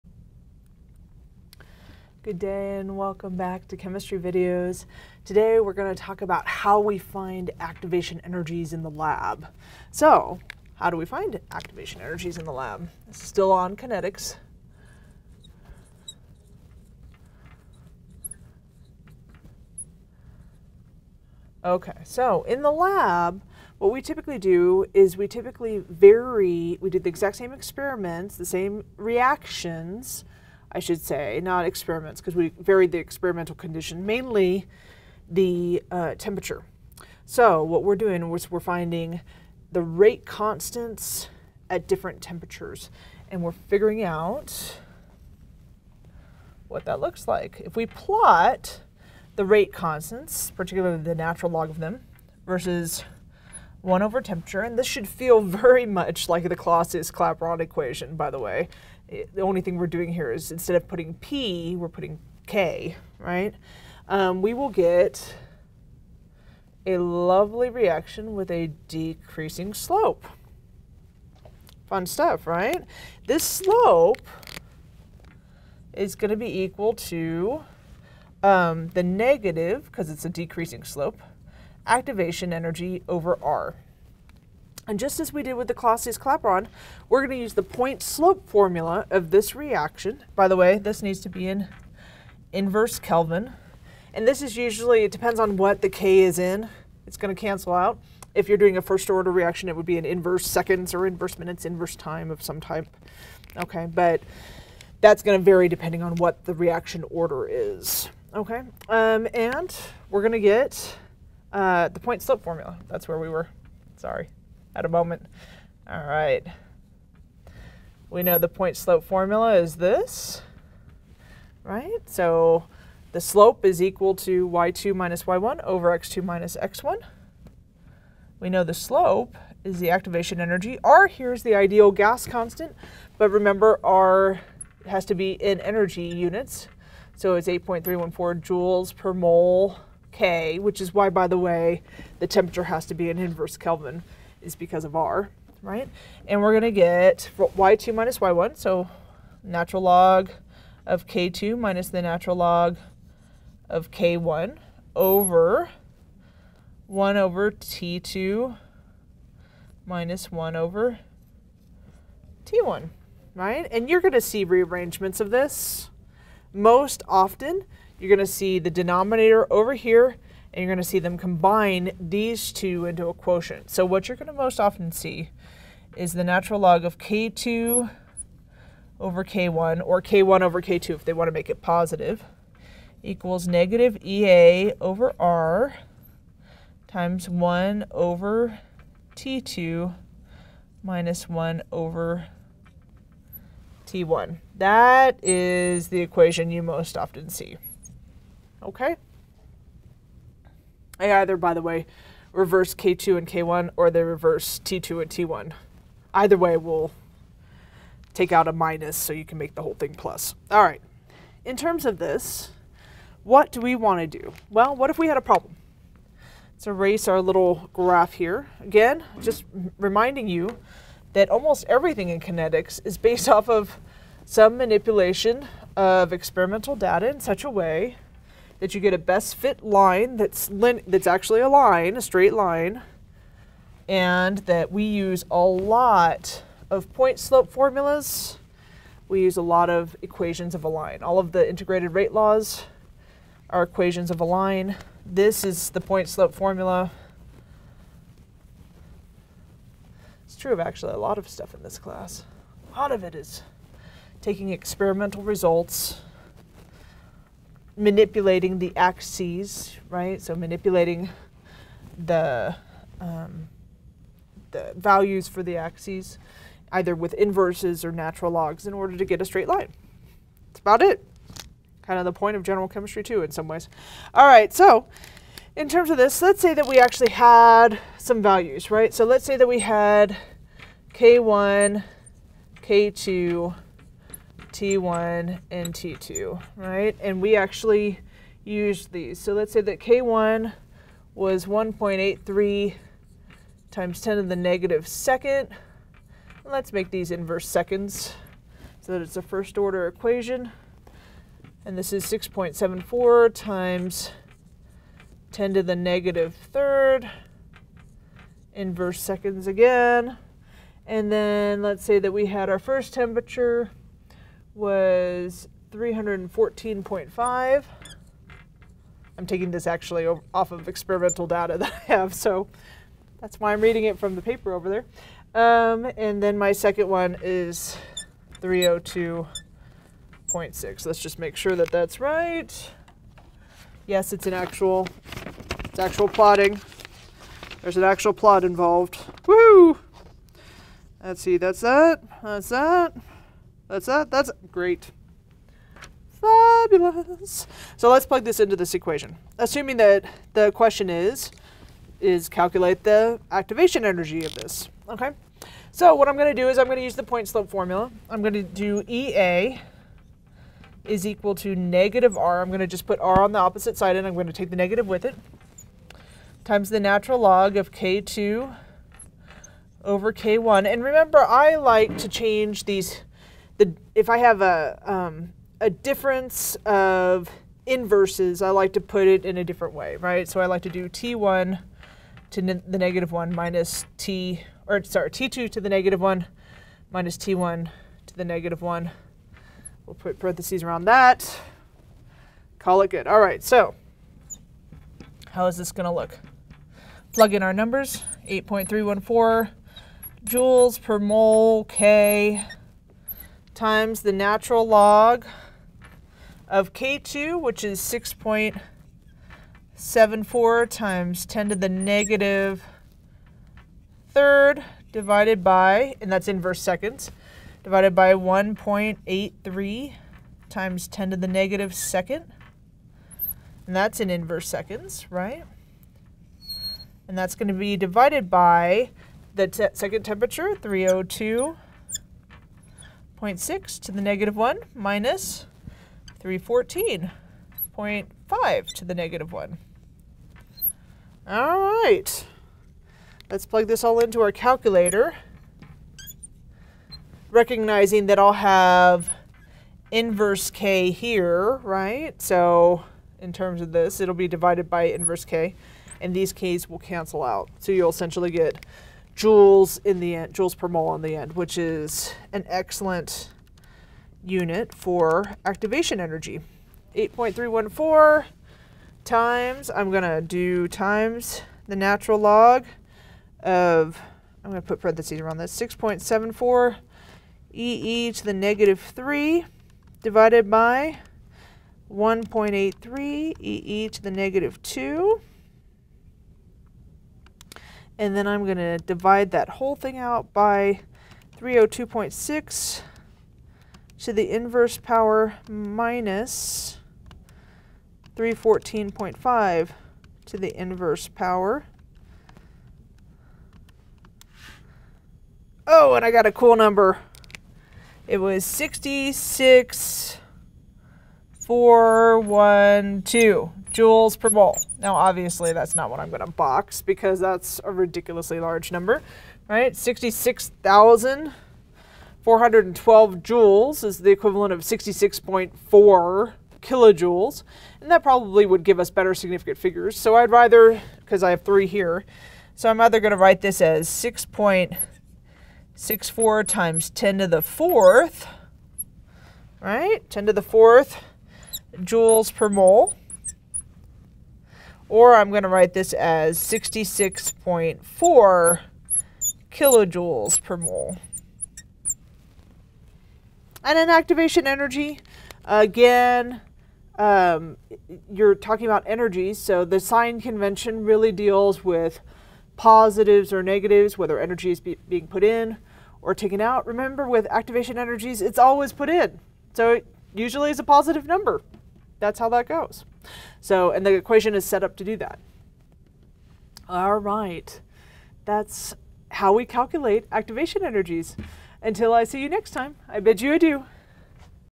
Kinetics: Calculating Activation Energies
This video discusses calculating the activation energy of a reaction using a graph that compares rate constants vs. temperature manipulations. Videos made by Clarissa Sorensen-Unruh on CNM Glass.